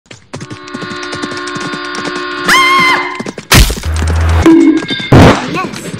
Girl falls down from the rope between trees into a hole but crane rescues her
Girl falls down from the rope between trees into a hole but crane rescues her near railway track